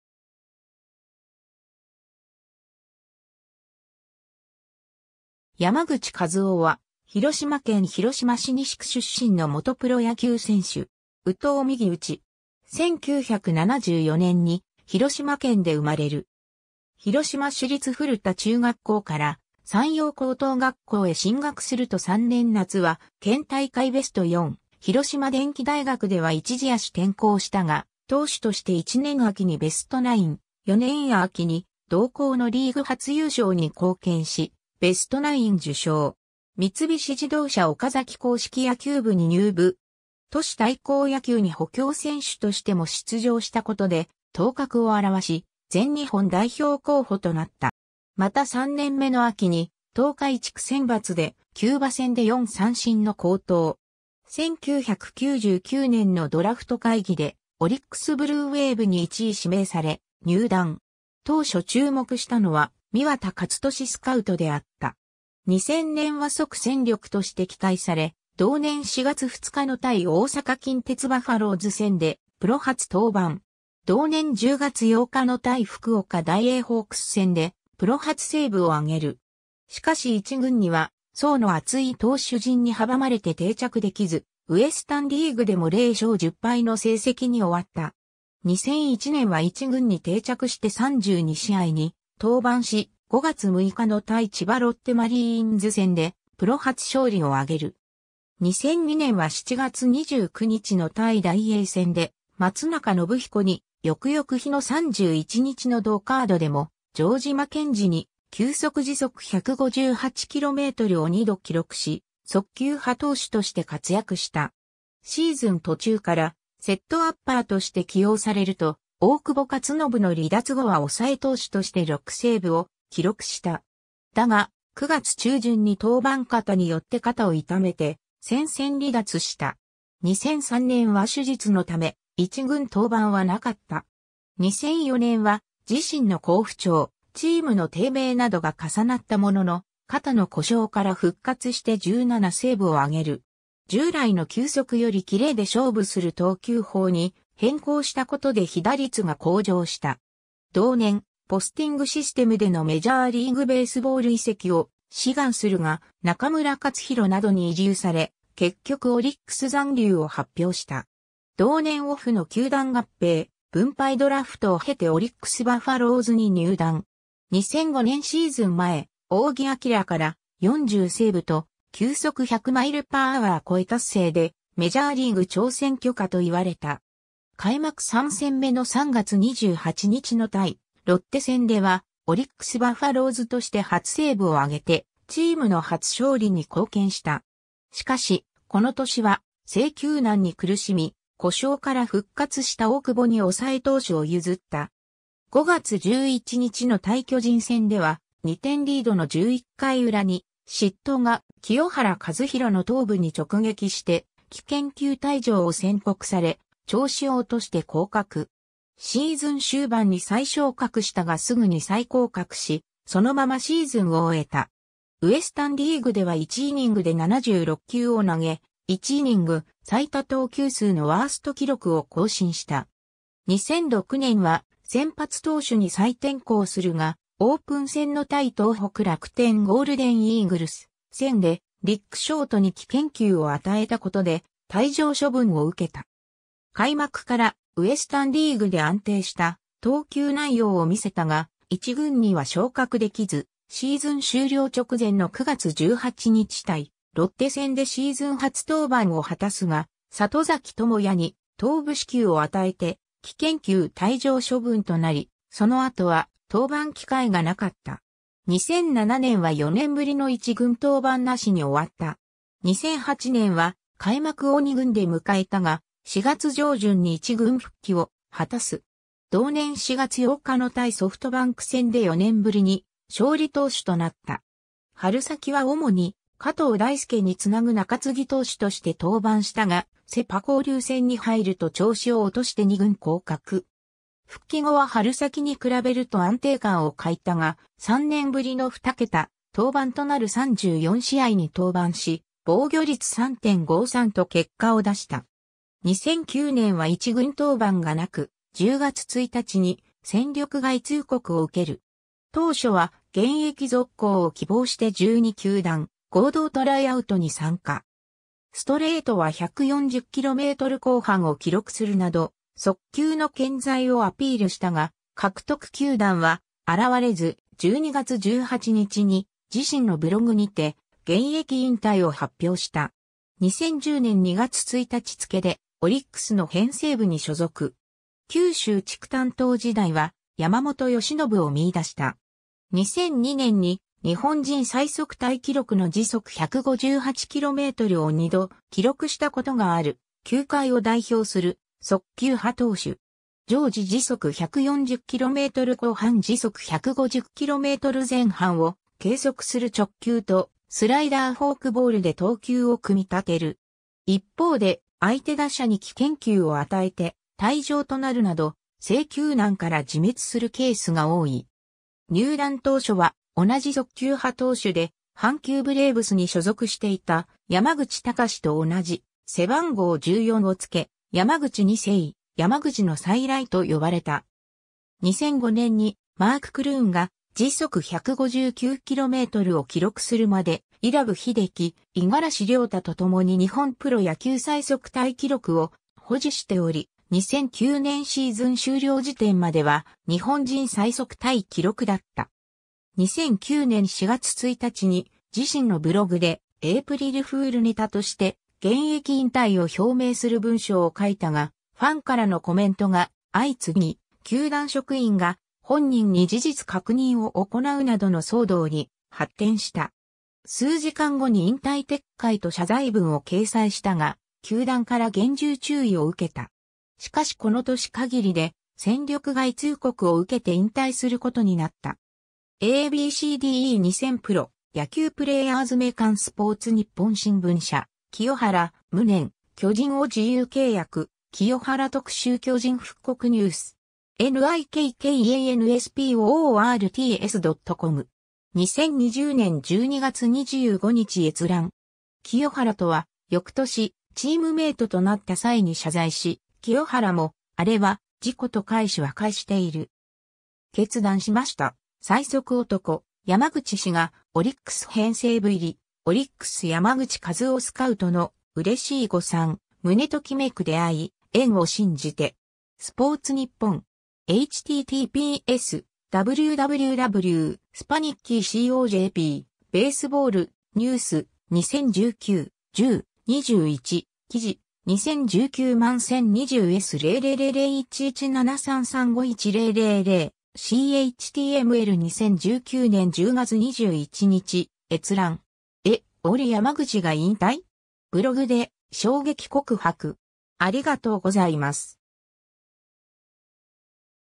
山口和男 (野球)
山口 和男（やまぐち かずお、1974年5月11日 - ）は、広島県広島市西区出身の元プロ野球選手（投手）。
右投右打。
 1974年に広島県で生まれる。
 広島市立古田中学校から山陽高等学校へ進学すると3年夏は県大会ベスト4、広島電機大学では一時野手転向したが、投手として1年秋にベストナイン、4年秋に同校のリーグ初優勝に貢献しベストナイン受賞。
三菱自動車岡崎硬式野球部に入部。
都市対抗野球に補強選手としても出場したことで頭角を現し、全日本代表候補となった。
また3年目の秋に東海地区選抜でキューバ戦で4三振の好投。
1999年のドラフト会議でオリックス・ブルーウェーブに1位指名（逆指名）され、入団。
当初注目したのは三輪田勝利スカウトであった。
 2000年は即戦力として期待され、同年4月2日の対大阪近鉄バファローズ戦（グリーンスタジアム神戸）でプロ初登板。
同年10月8日の対福岡ダイエーホークス戦（グリーンスタジアム神戸）でプロ初セーブを挙げる。
しかし一軍には層の厚い投手陣に阻まれて定着できず、ウエスタン・リーグでも0勝10敗の成績に終わった。
 2001年は一軍に定着して32試合に登板し、5月6日の対千葉ロッテマリーンズ戦（千葉マリンスタジアム）でプロ初勝利を挙げる。
 2002年は7月29日の対ダイエー戦で松中信彦に、翌々日の31日の同カードでも城島健司に球速158km/hを2度記録し、速球派投手として活躍した。
シーズン途中からセットアッパーとして起用されると、大久保勝信の離脱後は抑え投手として6セーブを記録した。
だが、9月中旬に登板過多によって肩を痛めて戦線離脱した。
 2003年は手術のため、一軍登板はなかった。
 2004年は自身の好不調、チームの低迷などが重なったものの、肩の故障から復活して17セーブを挙げる。
従来の球速よりキレで勝負する投球法に変更したことで被打率が向上した。
同年、ポスティングシステムでのメジャーリーグベースボール移籍を志願するが、中村勝広などに慰留され、結局オリックス残留を発表した。
同年オフの球団合併・分配ドラフトを経てオリックス・バファローズに入団。
 2005年シーズン前、仰木彬から「『40セーブと球速100mph（約160.9km/h）超え達成』でメジャーリーグ挑戦許可」と言われた。
開幕3戦目の3月28日の対ロッテ戦では、オリックス・バファローズとして初セーブをあげてチームの初勝利に貢献した。
しかし、この年は制球難に苦しみ、故障から復活した大久保に抑え投手を譲った。
5月11日の対巨人戦（東京ドーム）では、2点リードの11回裏に、失投が清原和博の頭部に直撃して危険球退場を宣告され、調子を落として降格。
シーズン終盤に再昇格したがすぐに再降格し、そのままシーズンを終えた。
ウエスタン・リーグでは1イニングで76球を投げ、1イニング最多投球数のワースト記録を更新した。
 2006年は先発投手に再転向するが、オープン戦の対東北楽天ゴールデンイーグルス戦でリック・ショートに危険球を与えたことで退場処分を受けた。
開幕からウエスタン・リーグで安定した投球内容を見せたが、一軍には昇格出来ず、シーズン終了直前の9月18日対ロッテ戦でシーズン初登板を果たすが、里崎智也に頭部死球を与えて危険球退場処分となり、その後は登板機会がなかった。
 2007年は4年ぶりの一軍登板なしに終わった。
 2008年は開幕を二軍で迎えたが、4月上旬に一軍復帰を果たす。
同年4月8日の対ソフトバンク戦で4年ぶりに勝利投手となった。
春先は主に加藤大輔につなぐ中継ぎ投手として登板したが、セ・パ交流戦に入ると調子を落として二軍降格。
復帰後は春先に比べると安定感を欠いたが、3年ぶりの2桁登板となる34試合に登板し、防御率3.53と結果を出した。
 2009年は一軍登板がなく、10月1日に戦力外通告を受ける。
当初は現役続行を希望して12球団合同トライアウトに参加。
ストレートは140km後半を記録するなど速球の健在をアピールしたが獲得球団は現れず、12月18日に自身のブログにて現役引退を発表した。
 2010年2月1日付でオリックスの編成部（スカウト）に所属。
九州地区担当時代は、山本由...